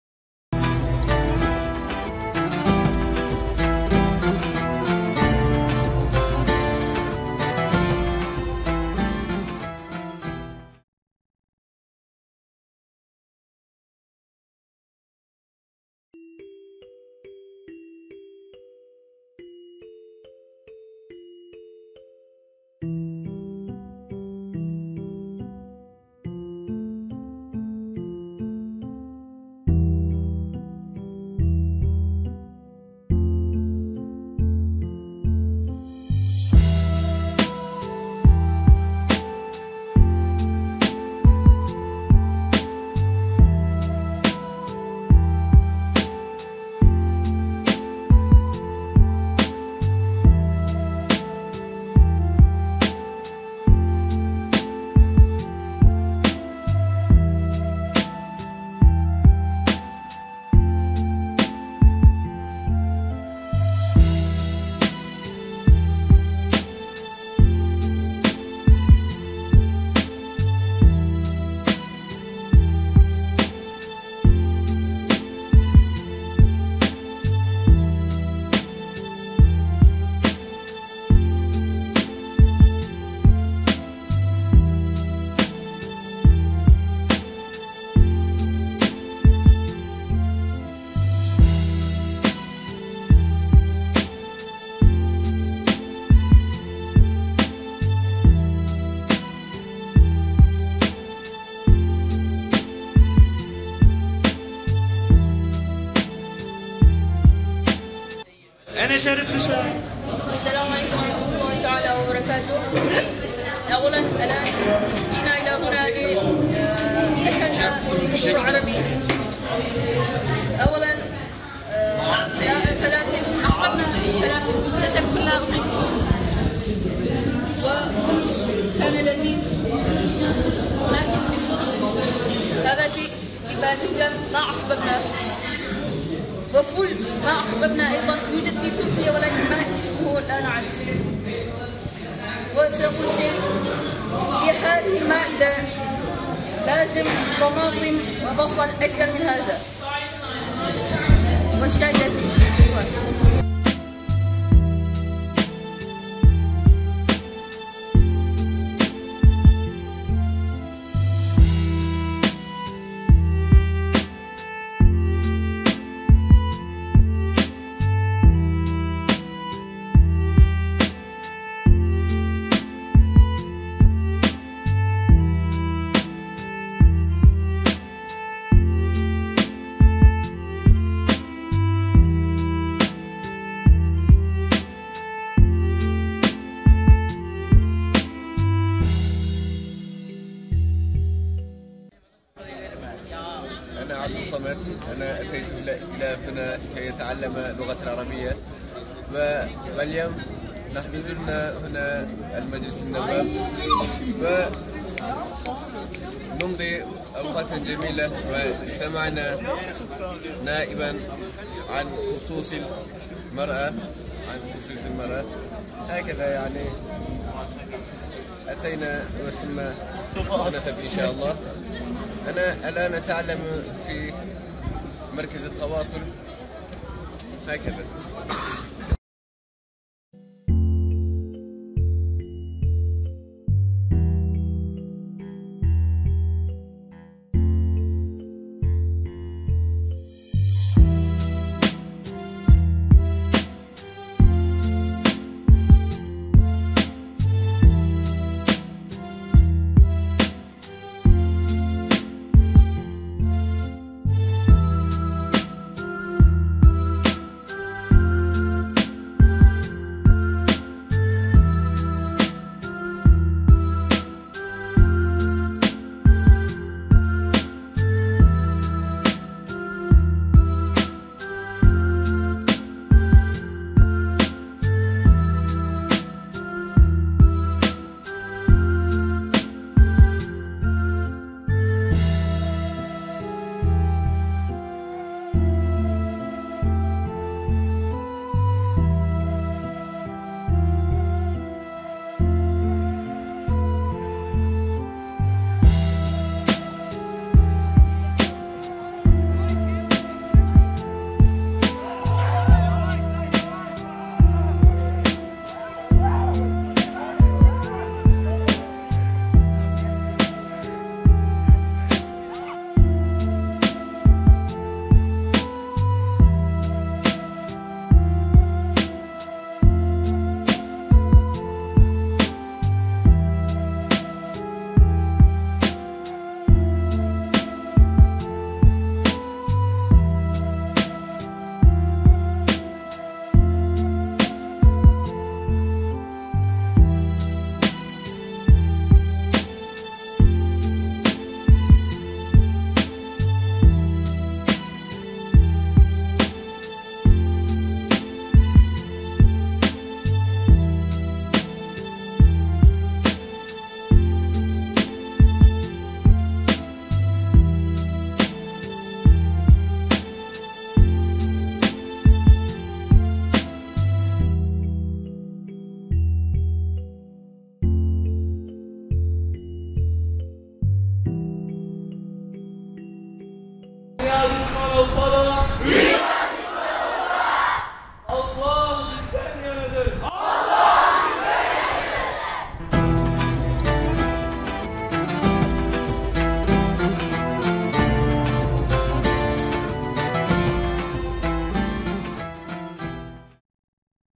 İTO ŞEHİT RAGIP ALİ BİLGEN AİHL ÜRDÜN KAMPI/فعاليات طلاب الفوج الثاني لمركز تواصل الحضارات
İstanbul Ticaret Odası Şehit Ragıp Ali Bilgen Anadolu İmam Hatip Lisesi Ürdün Gezisi ve Faaliyetleri...
 الاردن عمان  فعاليات طلاب الفوج الثاني لمركز تواصل الحضارات